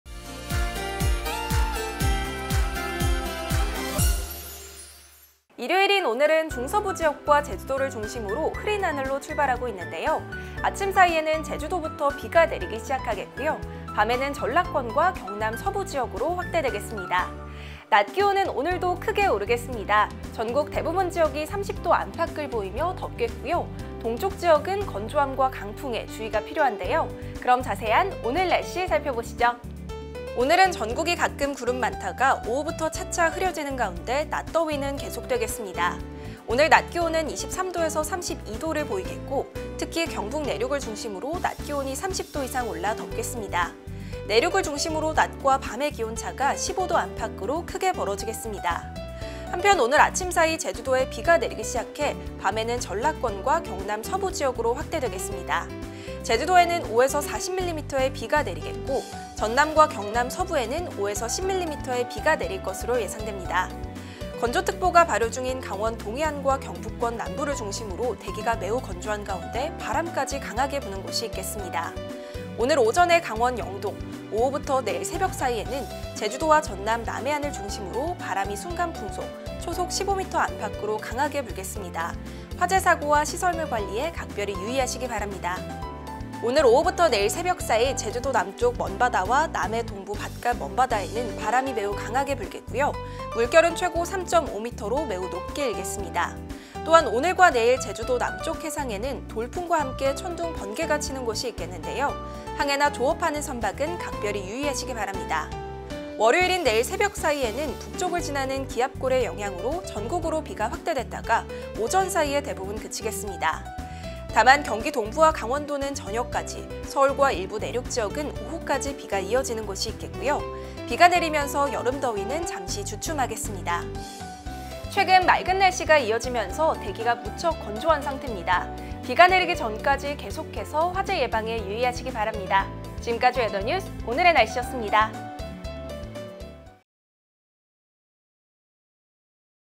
[웨더뉴스] 오늘의 날씨 (5월 29일 09시 기준)
일요일인 오늘은 중서부 지역과 제주도를 중심으로 흐린 하늘로 출발하고 있는데요.

아침 사이에는 제주도부터 비가 내리기 시작하겠고,

밤에는 전라권과 경남 서부 지역으로 확대되겠습니다.

한편, 낮 기온은 오늘도 크게 오르겠습니다.

전국 대부분 지역이 30도 안팎을 보이며 더울 것으로 보이는 가운데,

동쪽 지역은 건조함과 강풍에 주의가 필요하겠는데요.

자세한 정보는 영상에서 확인해 주세요~!!

오늘의 캐스터 : 모세원